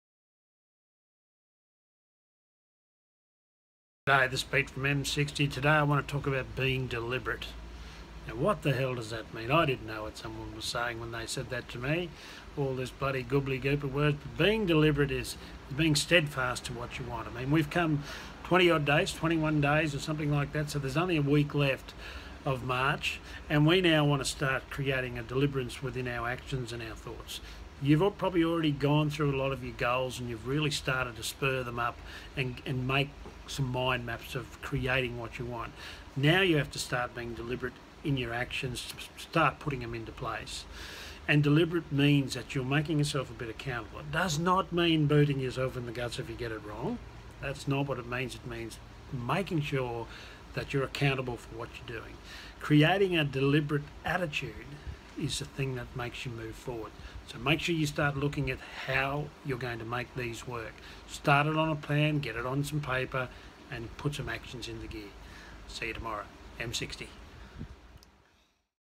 Peter Wheaton..How to Be Deliberate ...Oh2bwealthy.. Day (23)
Start making your actions deliberate and attach and emotion and your dream to create your success...